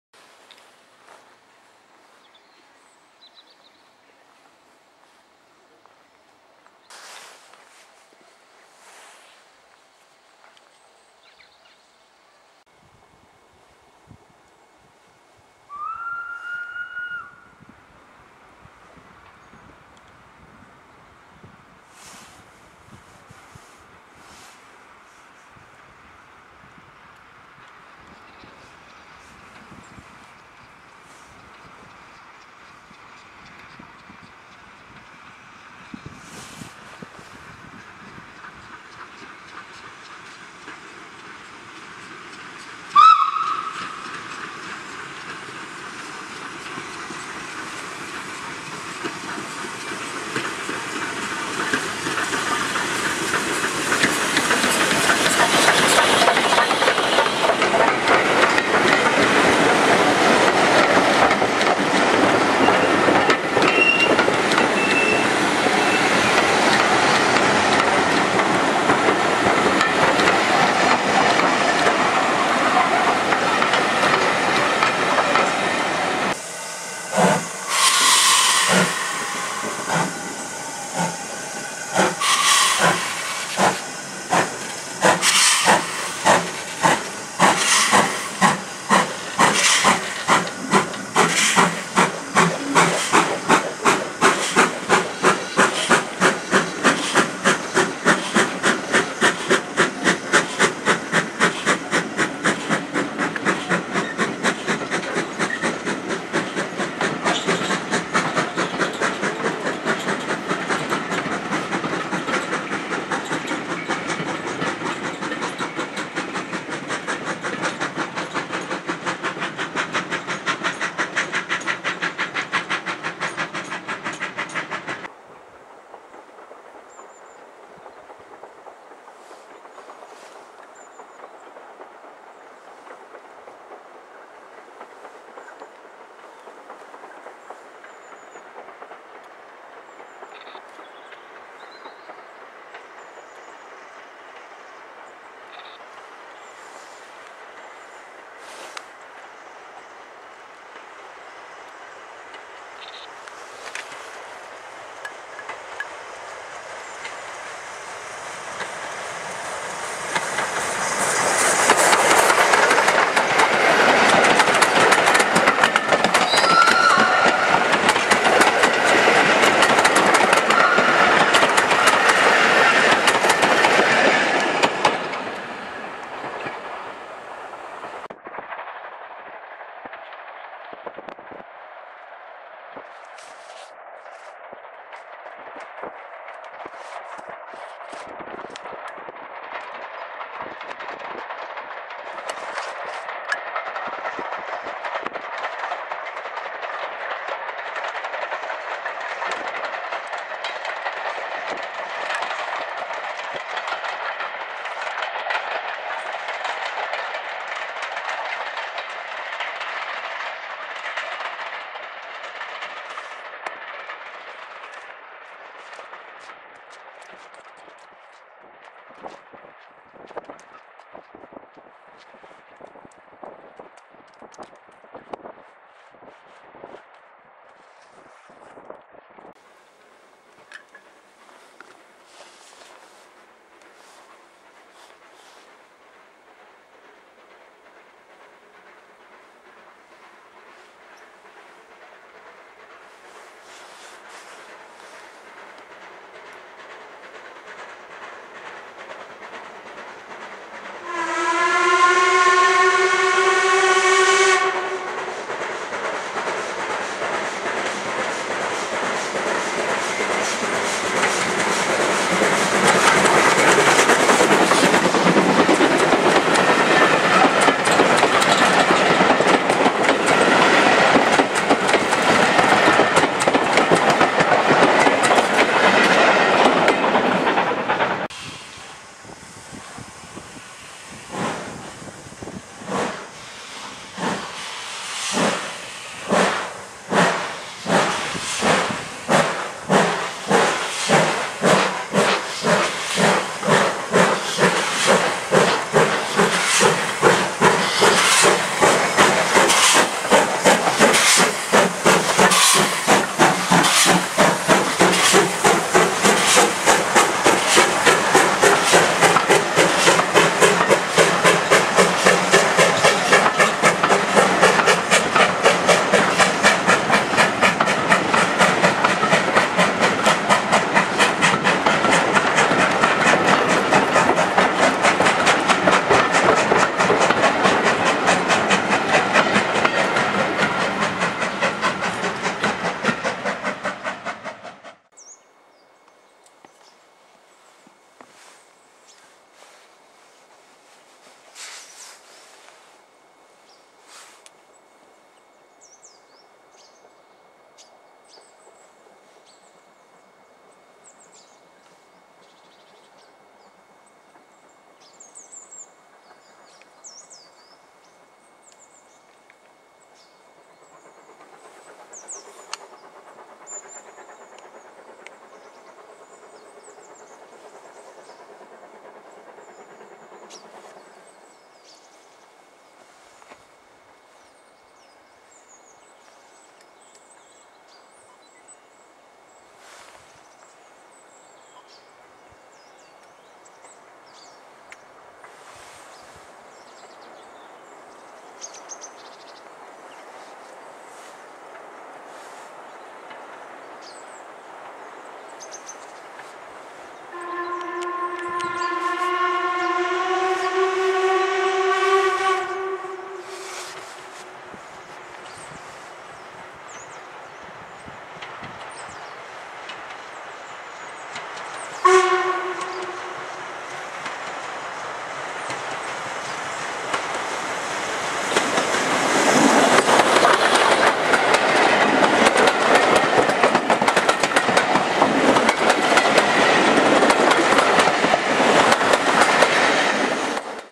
Strathspey Railway 2011
I spent a couple of days filming on The Strathspey Railway, and here are some of the better shots. Based around Boat Of Garten, the video sees Ivatt 2mt number 46512 "E. V. Cooper, Engineer" with two whistle during the week. The GNSR whistle offers a haunting, but beautiful sound andis a memorial to the savoir of the locomotive, who also purchased the whistle as the original was gone. The Stanier whistle is from under restoration black 5, no. 5025. I was hoping to film Caledonian 828, but its still at the Severn Valley Railway after its stunning preformance at their recent Autumn Steam Gala.